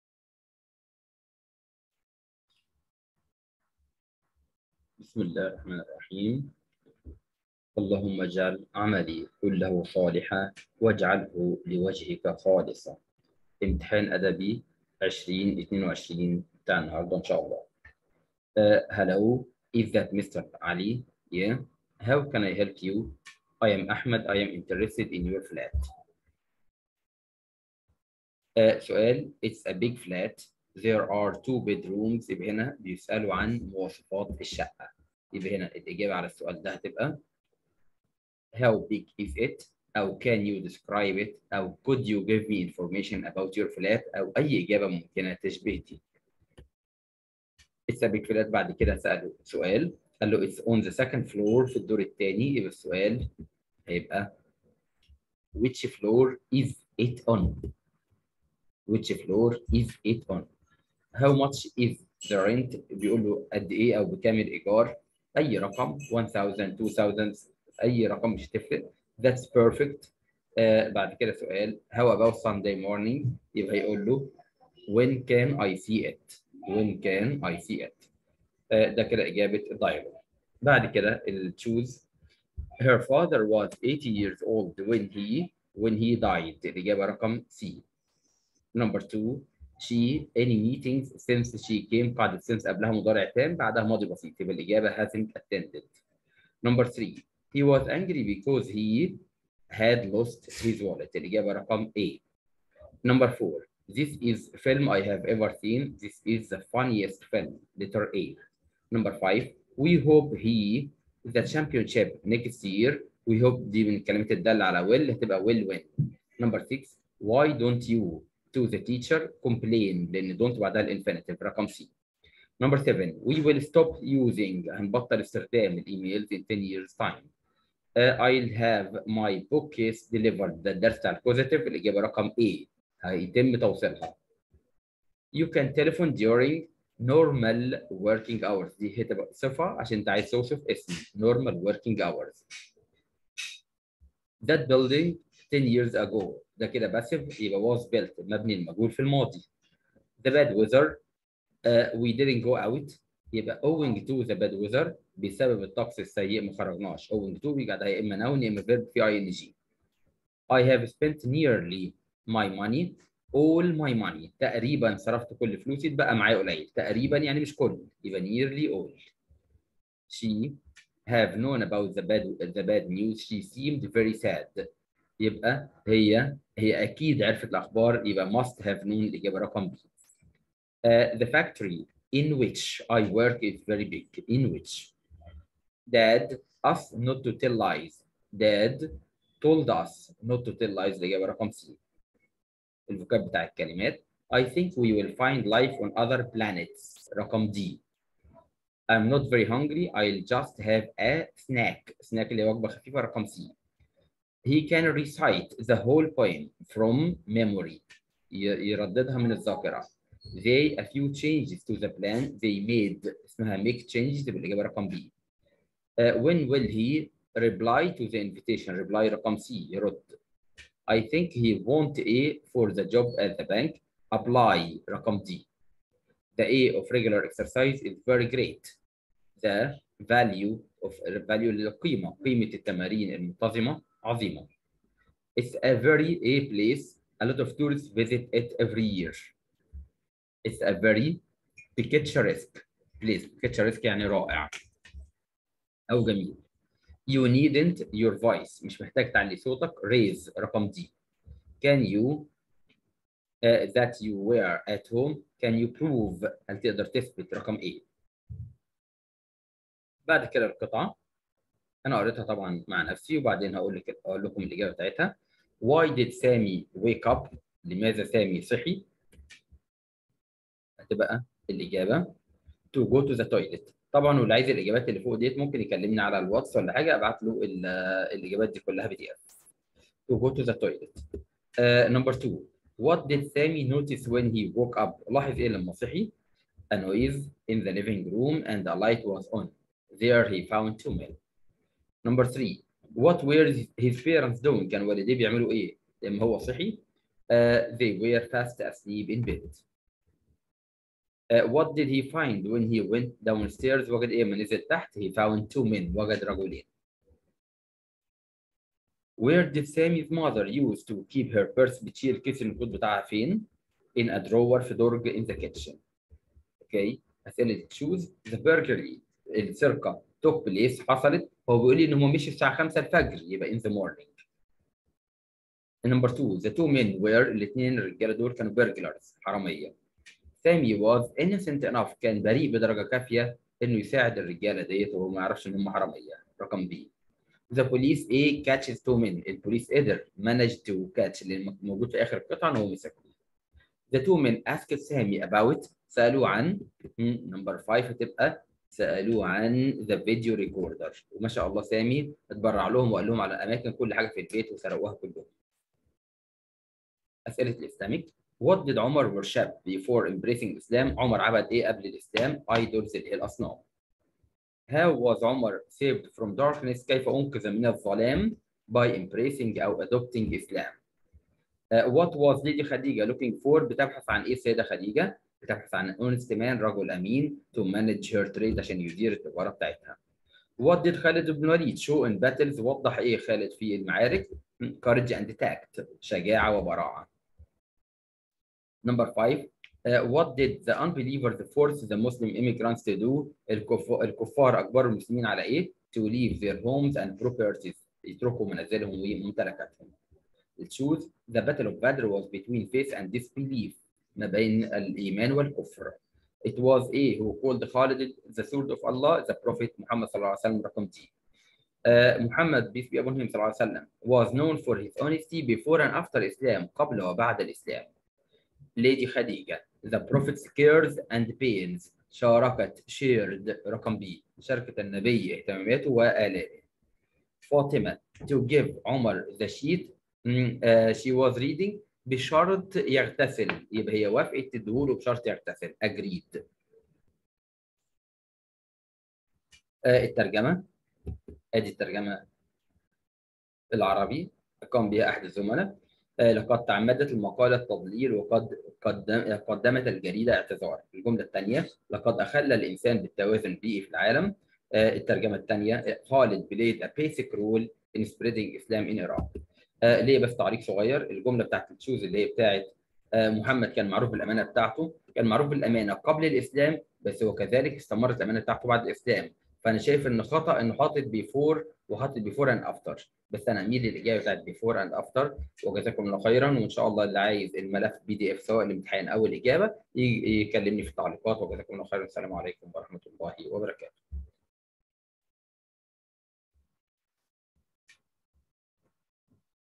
امتحان انجليزي ادبي ازهر 2022
حل اختبار اللغة الانجليزية للصف الثالث الثانوي ازهر ادبي 2022